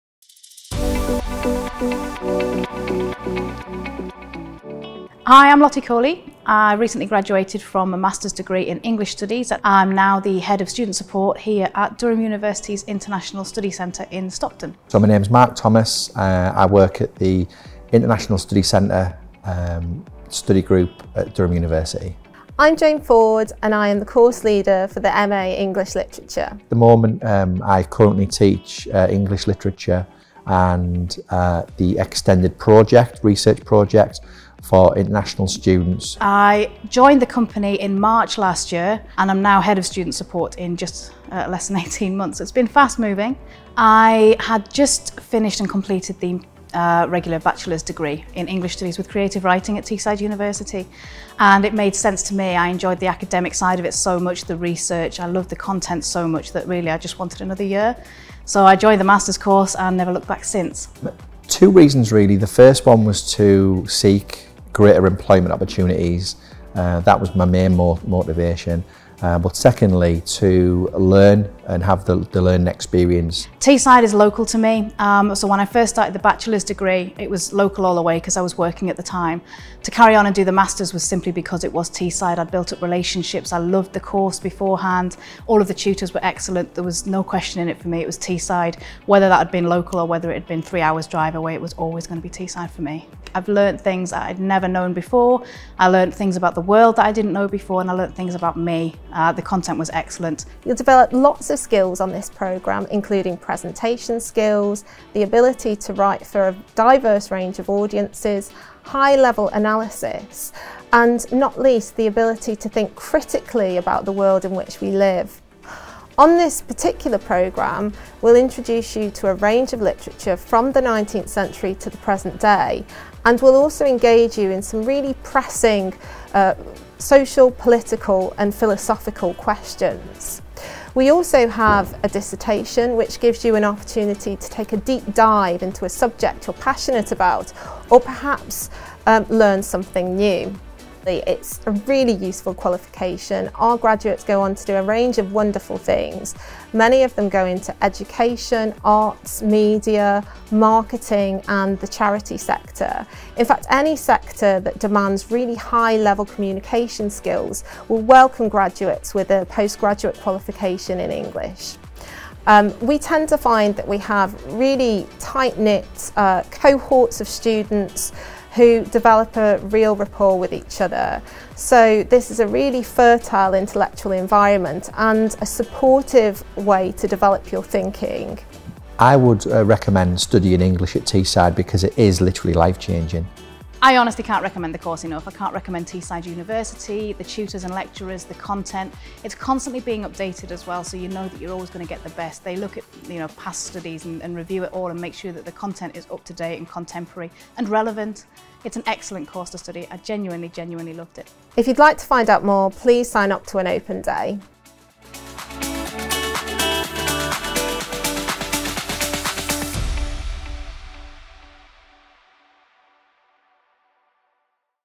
English Literature at Teesside
Find out why English graduates recommend the postgraduate course, how it helped them secure their roles, and there’s an insight from the course tutor.